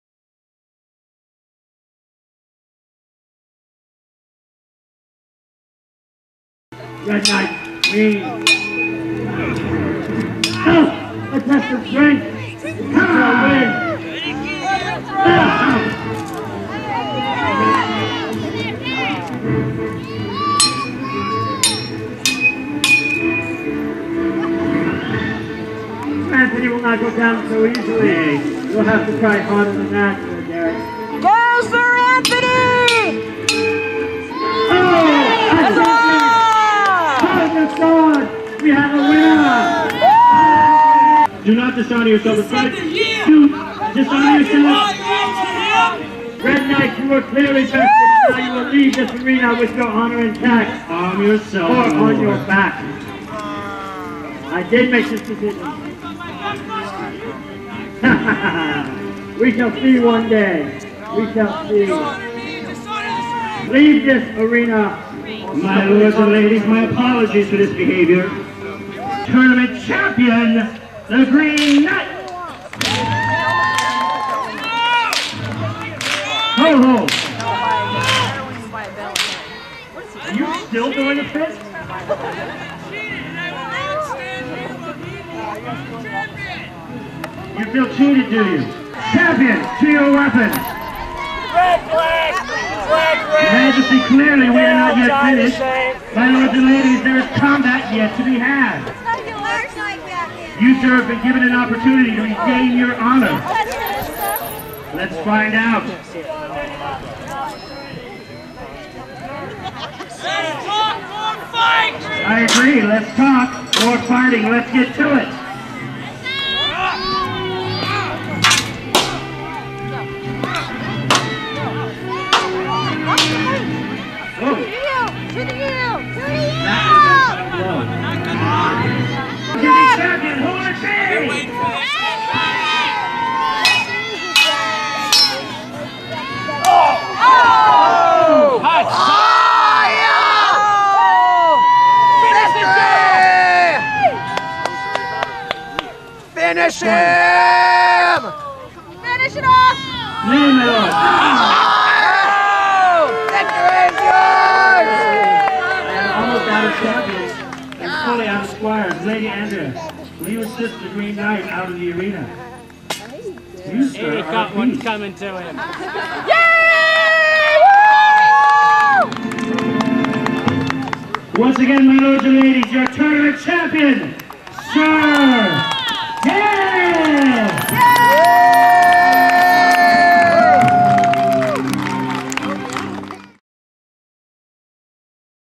The Red Knight II To Thee Yield
Sir Derek The Red Knight fights Sir Anthony and The Green Knight.